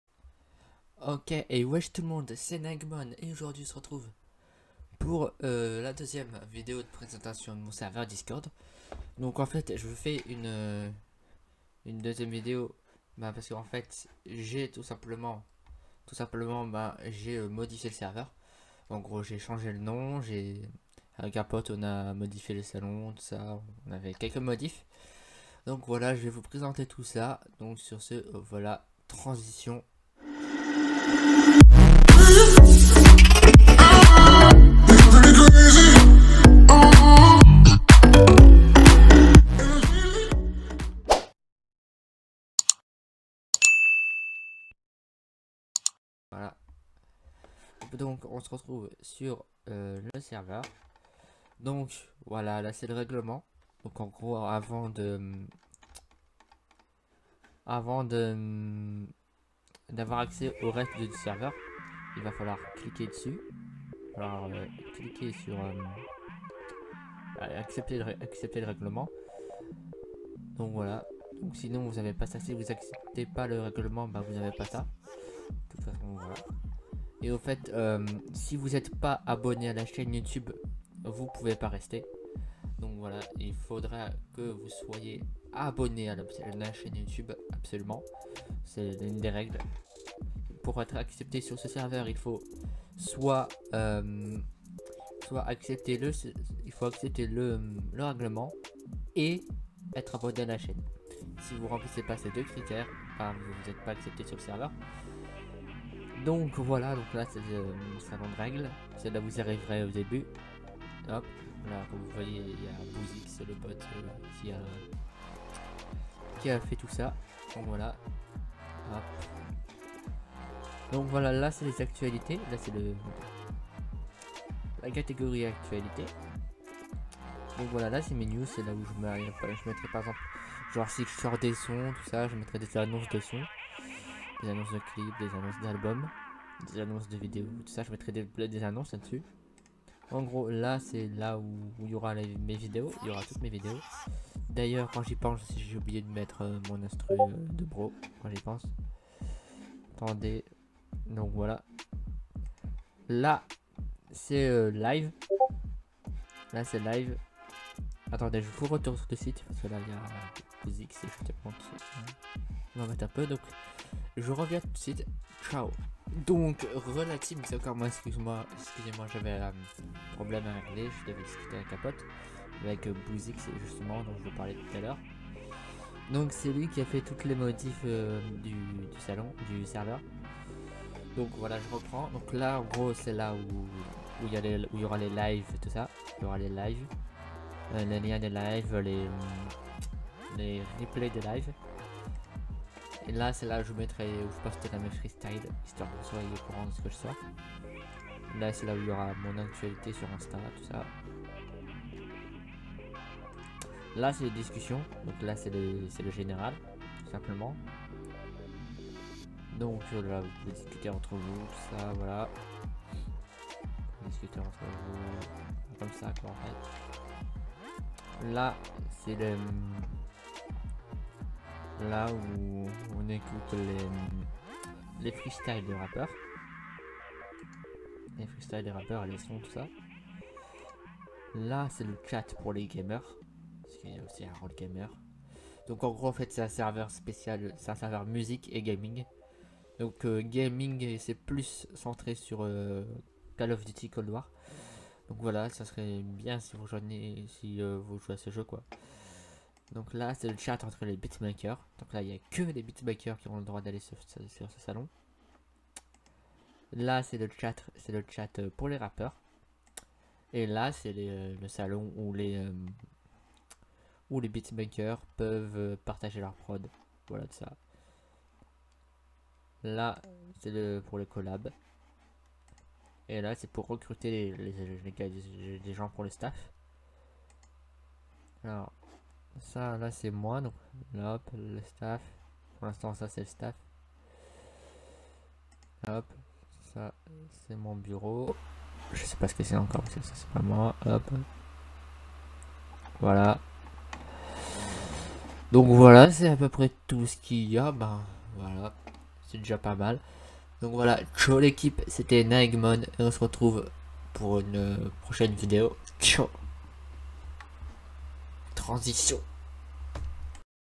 Présentation serveur discord (sole gang) 2/2
Wesh tout l'monde c'est Naegmon et aujourd'hui on se retrouve pour la présentation de mon serveur discord, le Sole Gang